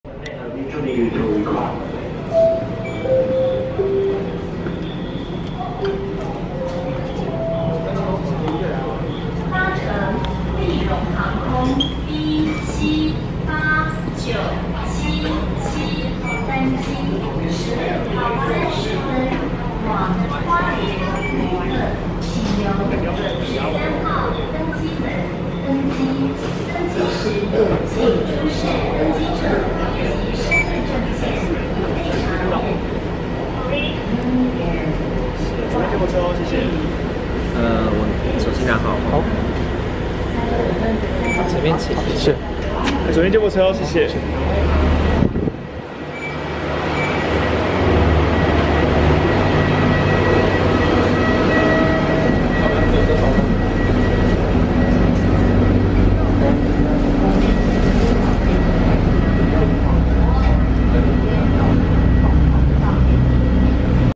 B78977 立榮航空台北松山機場登機 UNI AIR Taipei Songshan International Airport Boarding
2023/10/22
立榮航空 UNI AIR
台北松山(TSA)---花蓮(HUN)
登機門:13
航班:B78977
機型:ATR72-600
機身編號::B-17008
座位:10K
Push back:16:29
預定/實際起飛:16:30/16:39
預定/實際抵達:17:20/17:13
航程:34min